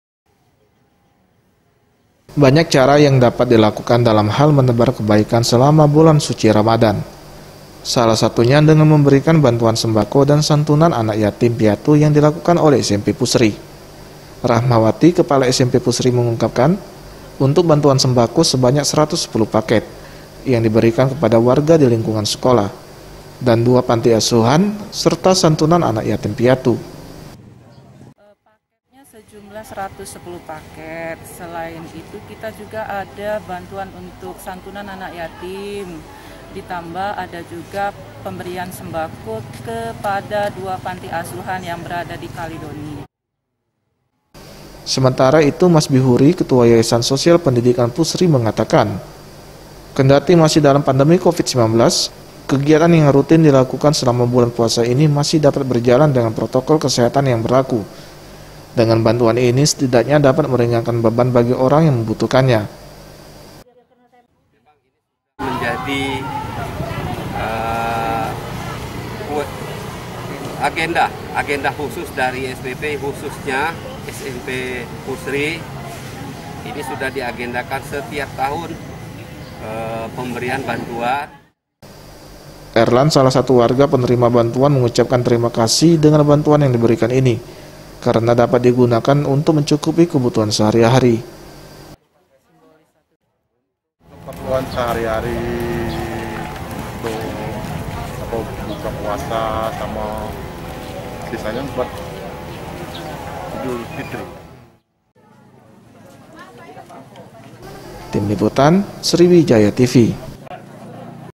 Berkah Ramadhan Berikan Bantuan Sembako Dan Santunan Anak Yatim Piatu
Banyak cara yang dapat dilakukan dalam hal menebar kebaikan selama bulan suci ramadhan , salah satunya dengan memberikan bantuan sembako dan santunan anak yatim  piatu,  yang dilakukan oleh Smp Pusri. Sebanyak 110 paket yang diberikan kepada warga di lingkungan sekolah   dan 2 panti asuhan, serta  santuanan anak yatim piatu.